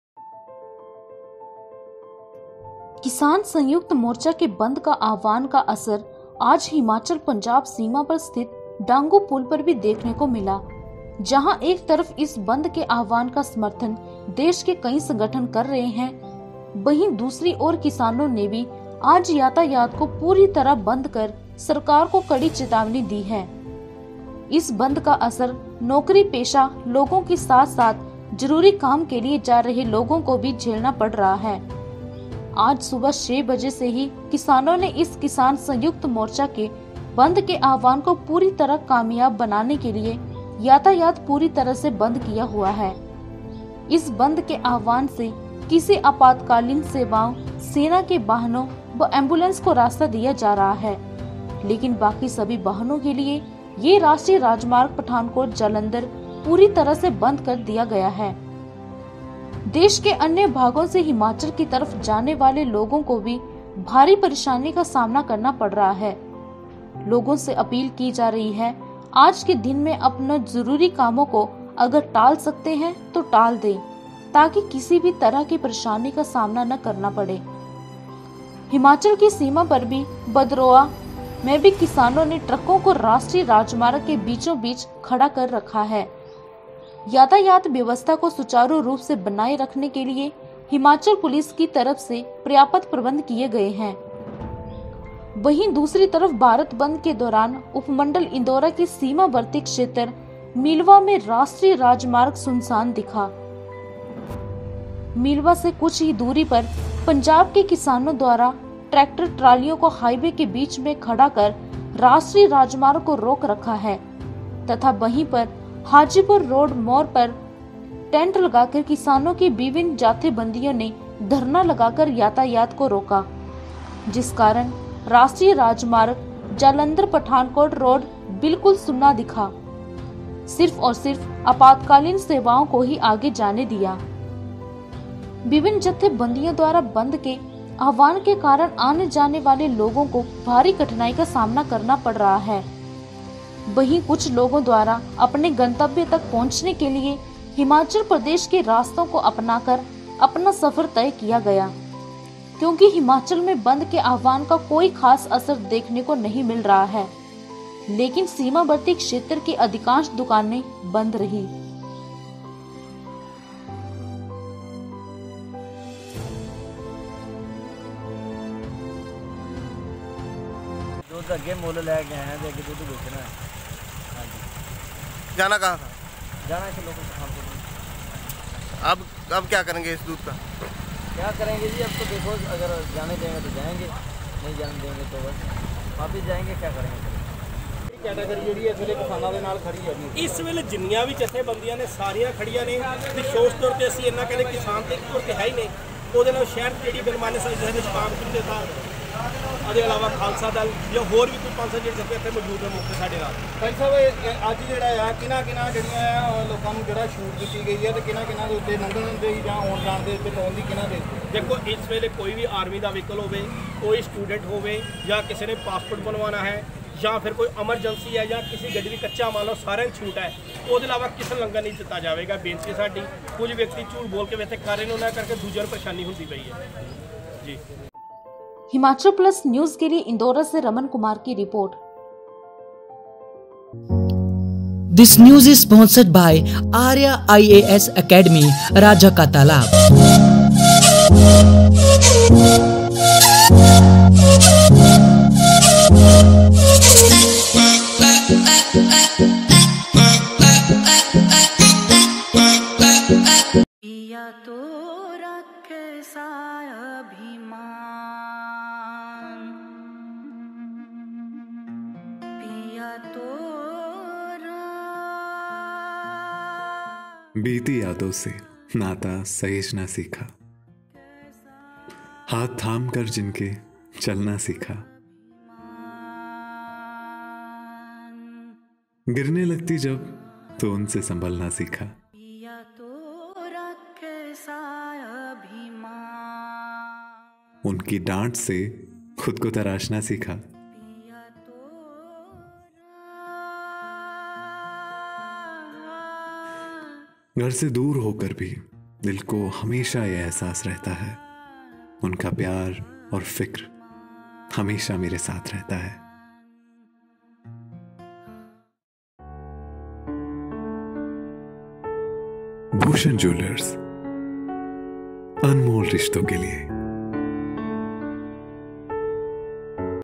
इंदौरा : हिमाचल पंजाब सीमा पर दिखा भारत बंद का असर
Himachal Plus News APP, Himachal No.1 Hindi News App

पंजाब सीमा पर दिखा भारत बंद का असर